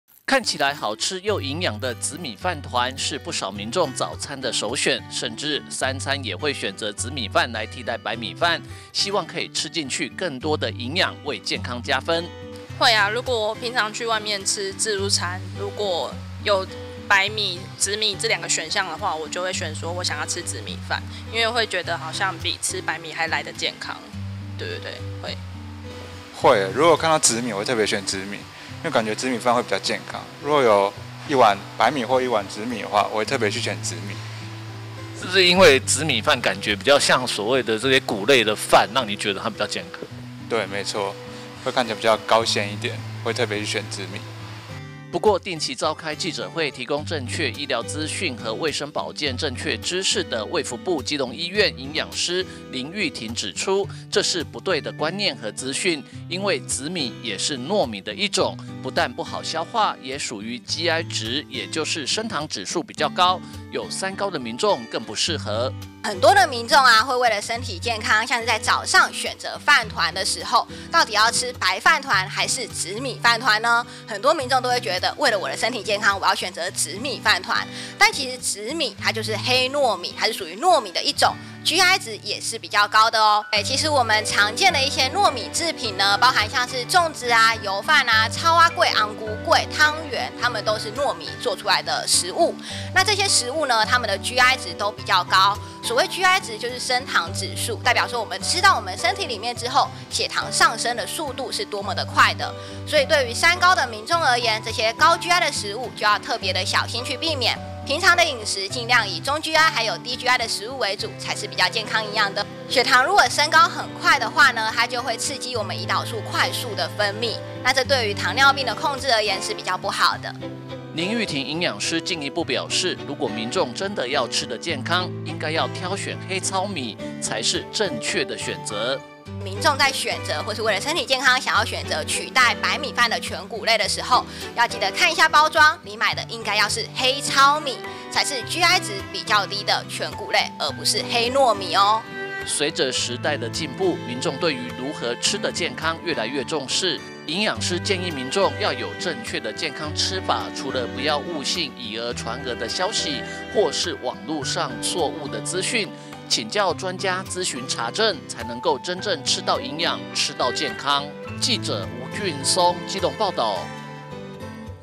基隆|衛福部基隆醫院|營養師|紫米|黑糙米|健康|糯米|假訊息|查證|高GI|低GI|升糖指數|三高|飯糰|吃紫米較健康？營養師建議查證避免誤信假訊息/吃紫米比較健康？營養師:紫米是糯米GI值高
吃紫米較健康？營養師建議查證避免誤信假訊息

吃紫米比較健康？營養師:紫米是糯米GI值高

(中嘉吉隆新聞記者吳俊松/基隆報導)許多民眾認為吃紫米飯比吃白米飯更健康，因此都會選擇吃紫米飯，不過衛福部基隆醫院營養師指出，這是不對的觀念和資訊，因為紫米也是糯米的一種，GI值，也就是升糖指數比較高，建議民眾在飲食上想要吃得更健康，應該向營養師等專業人員諮詢查證！

看起來好吃又營養的紫米飯糰，是不少民眾早餐的首選，甚至三餐也會選擇紫米飯來替代白米飯，希望可以吃進去更多的營養為健康加分！

定期召開記者會提供正確醫療資訊和衛生保健正確知識的衛福部基隆醫院營養師林鈺庭指出，這是不對的觀念和資訊，因為紫米也是糯米的一種，不但不好消化也屬於GI值也就是升糖指數比教高，有三高的民眾更不適合！

林鈺庭營養師進一步表示，如果民眾真的要吃得健康，應該選擇黑糙米才是正確的選擇！

隨著時代的進步，民眾對於如何吃得健康越來越重視，營養師建議民眾要有正確的健康吃法，除了不要誤信以訛傳訛或是網路上錯誤的資訊，請教專家諮詢查證，才能真正吃到營養吃到健康！